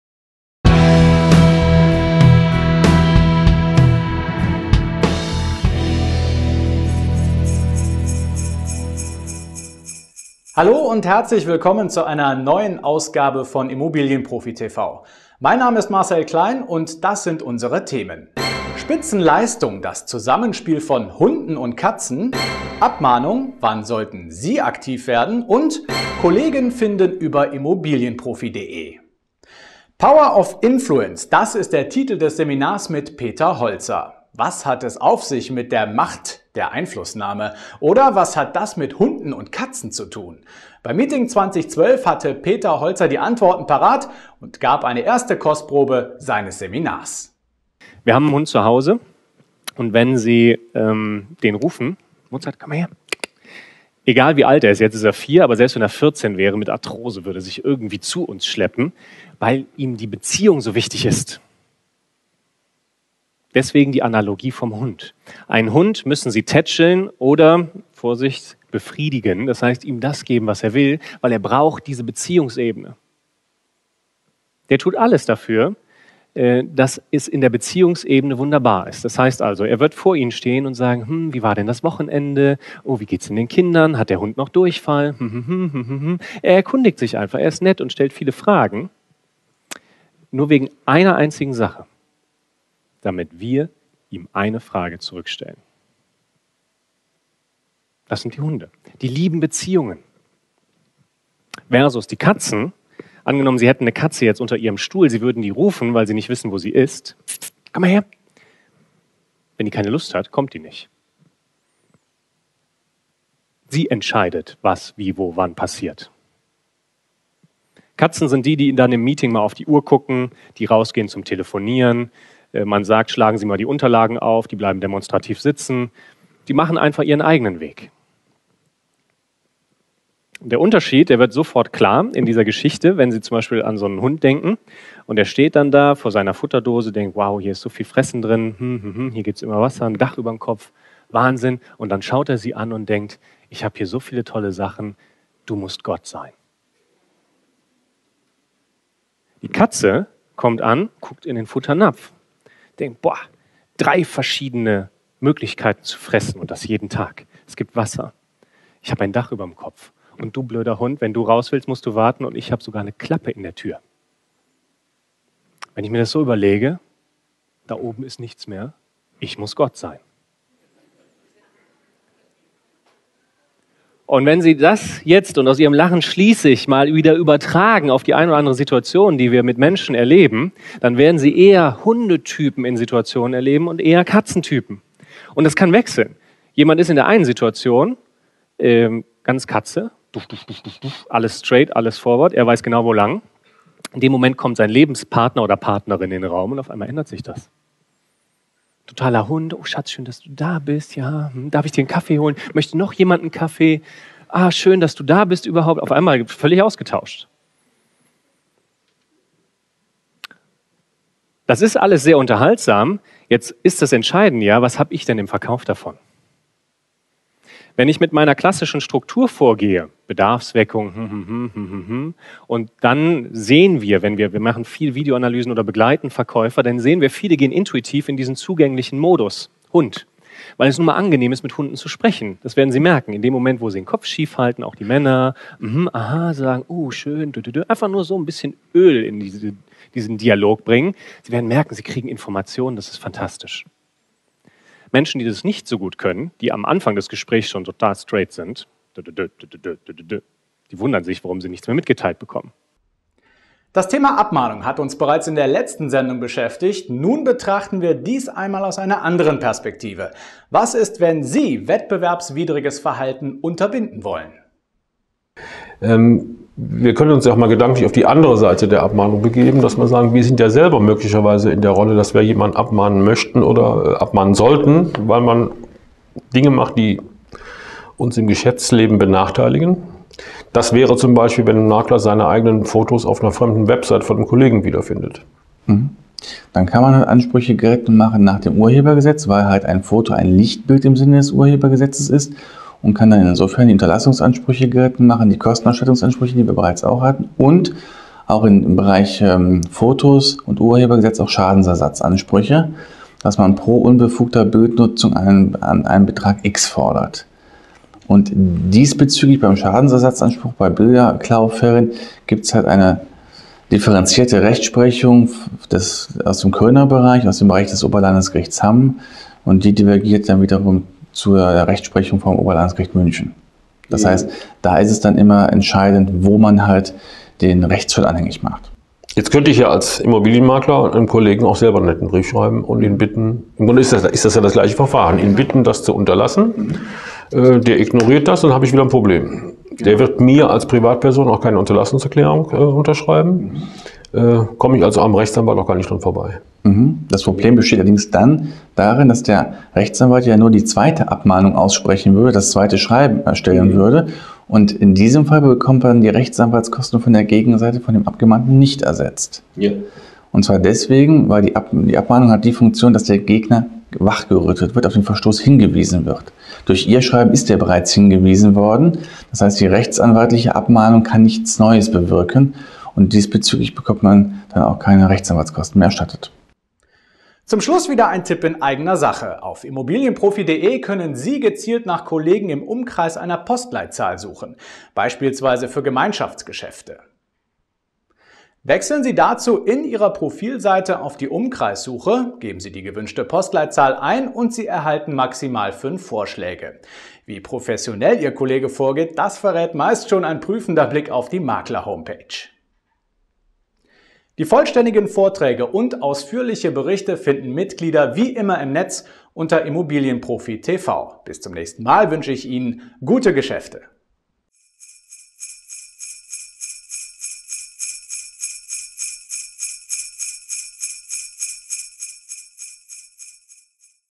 IMMOBILIEN-PROFI TV-Show 51
Die Themen dieser Show:

* Peter Holzer: Das Zusammenspiel zwischen Hunden & Katzen
* Interview Guido Vierkötter: Abmahnung - Wettbewerbswidriges Verhalten unterbinden
* In eigener Sache: Umkreissuche